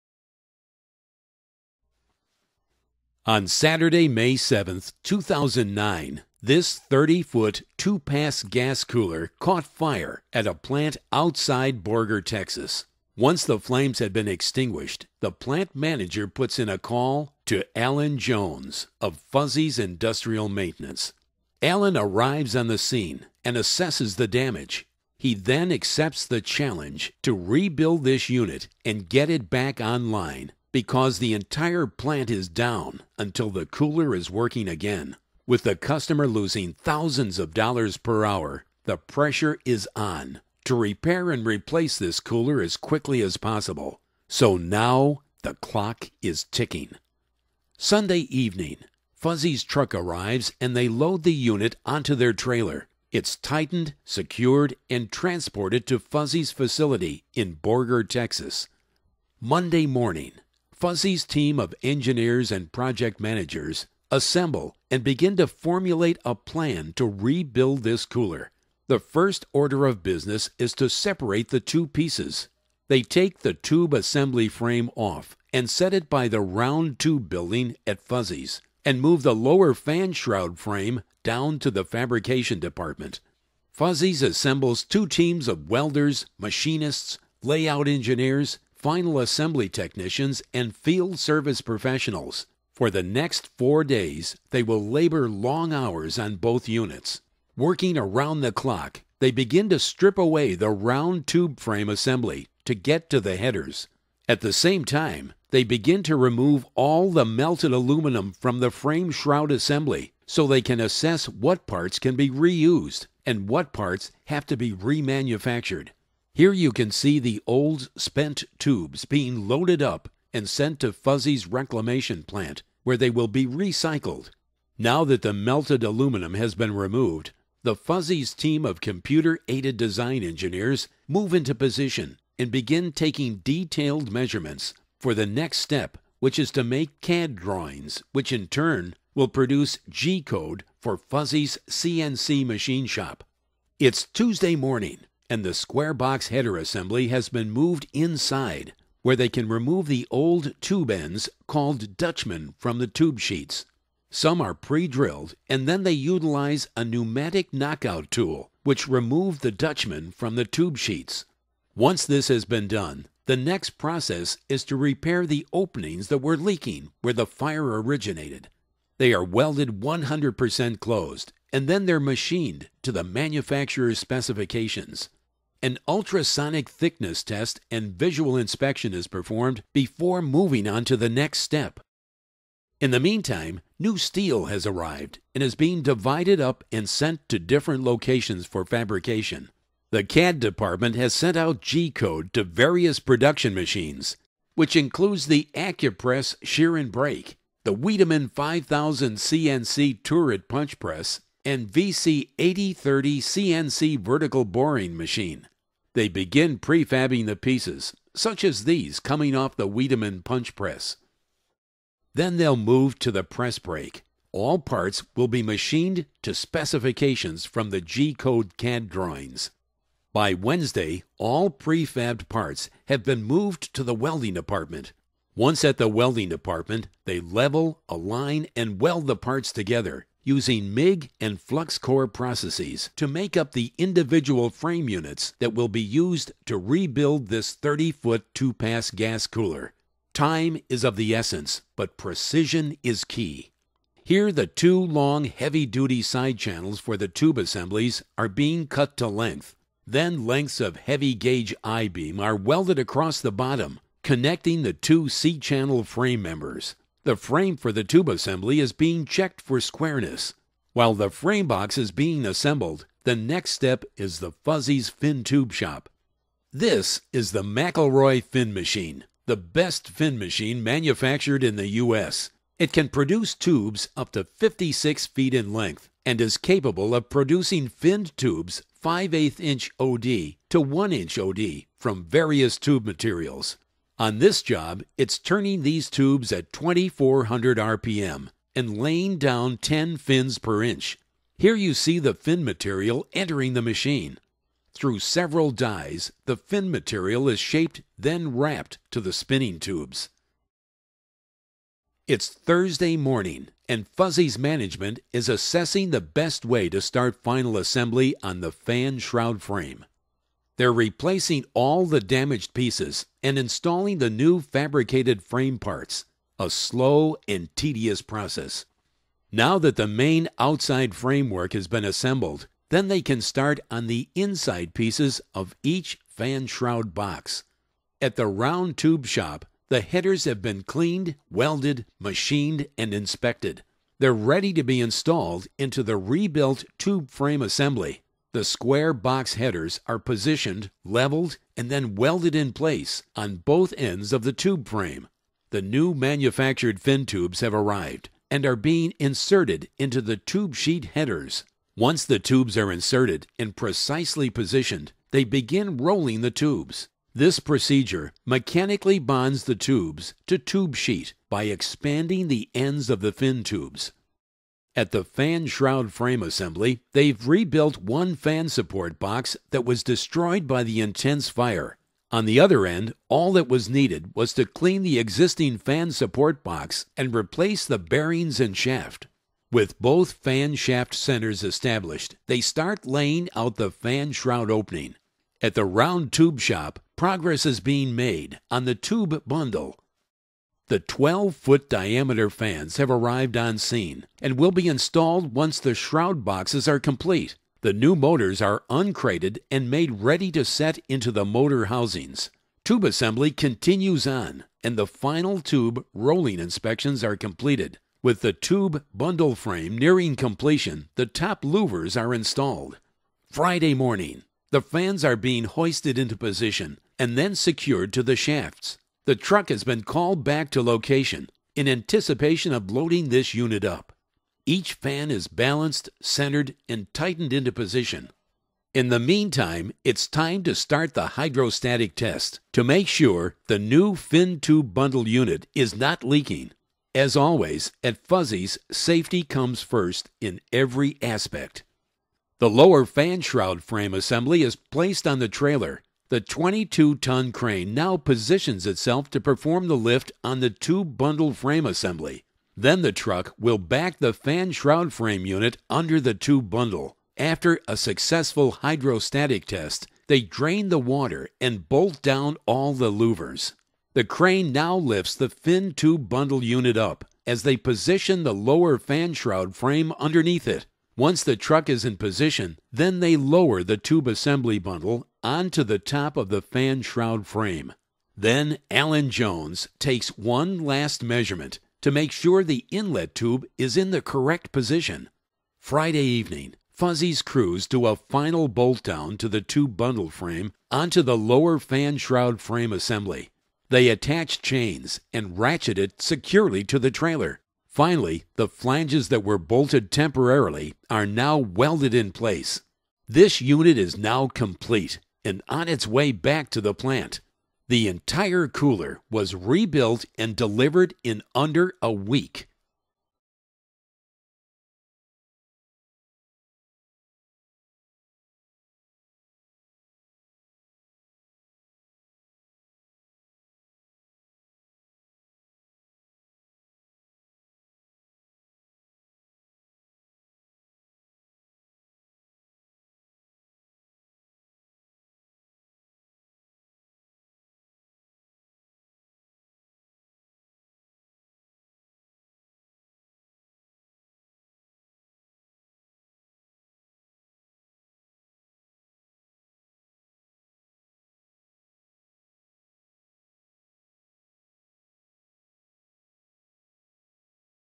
Itech Design marketing dvd of Fuzzy's Radiator gas cooler .wmv
A industrial marketing DVD using video production, 3D animation, still photography, and motion graphics to illustrate the speed and efficiency of Fuzzys Radiator in Borger Texas to rebuild a two pass gas cooler destroyed by fire.